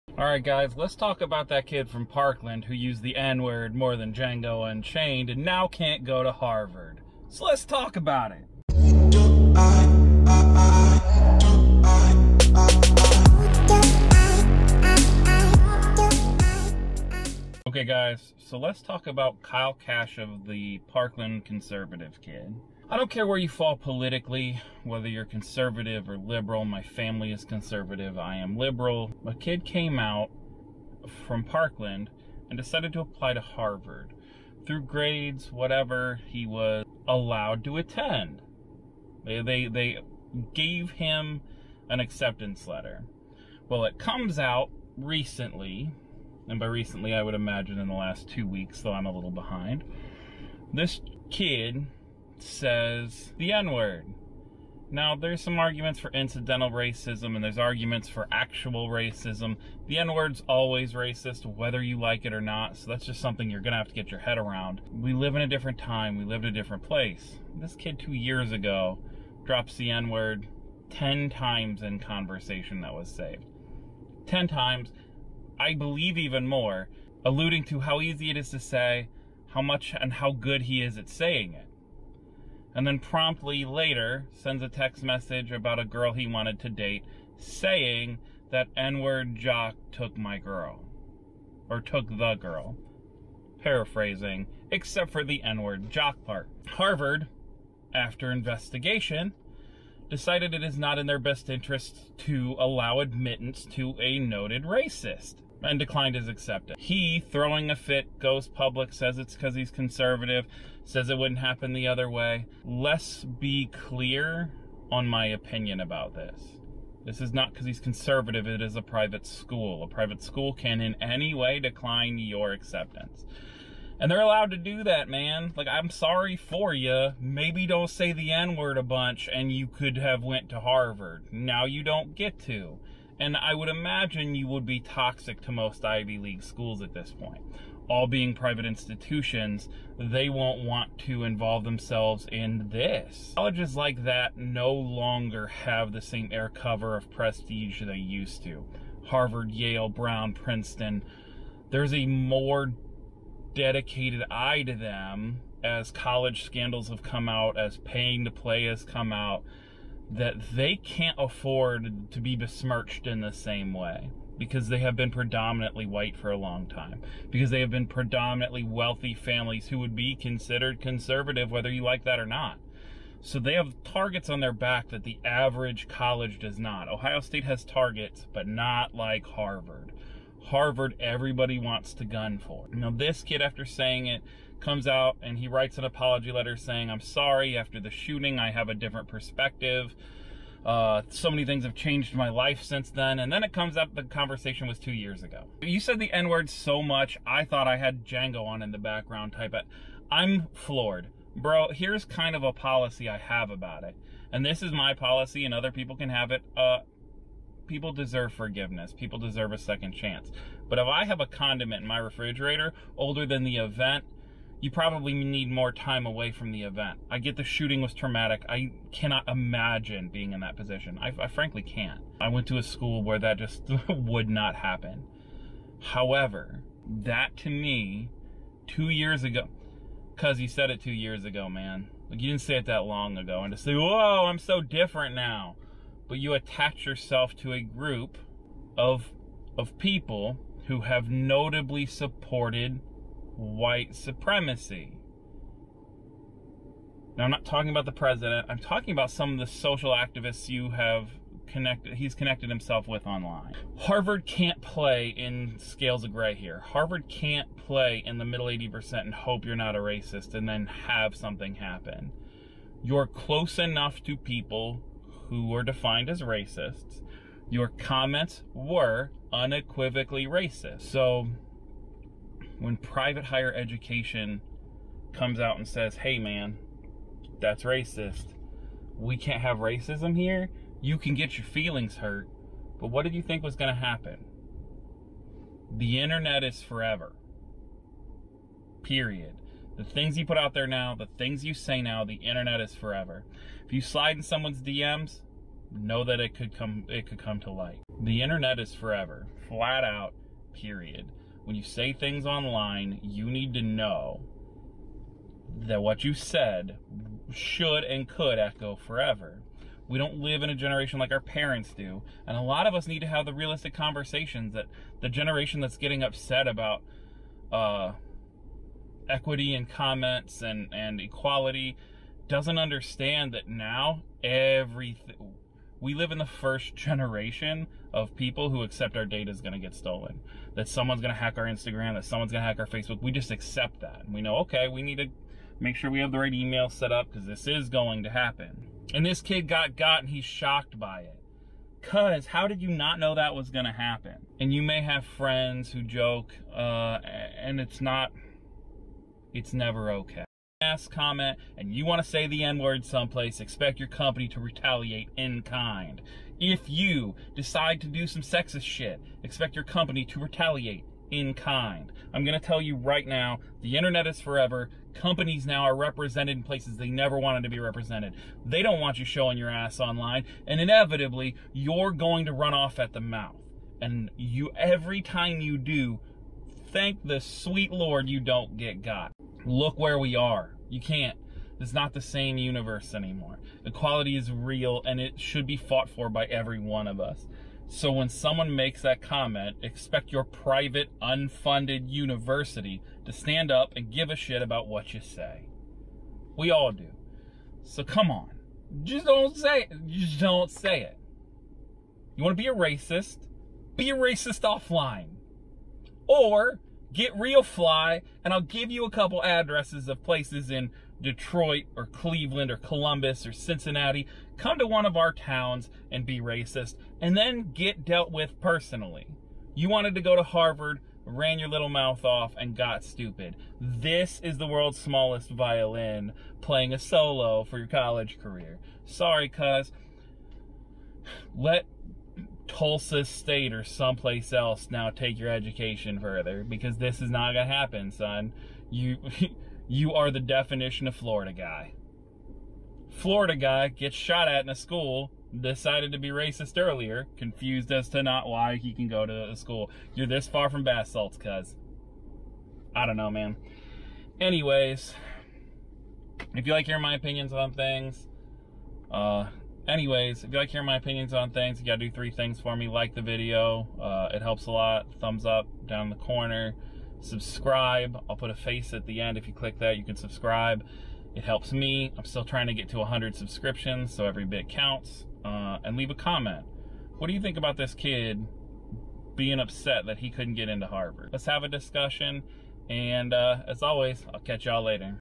Kyle Kashuv and Harvard
What's up guys! Kyle Kashuv this week had his acceptance taken back by Harvard after evidence of multiple racist comments. This boy from Marjory Stoneman was a survivor of the Parkland Shooting, and outspoken since the event. Unfortunately, he got caught being a racist. Here's my opinion.

GEAR:
Camera : Nikon Z7
Lens : Nikon 35mm 1.8s
Software: Final Cut Pro X
Mic : Rode -Video Pro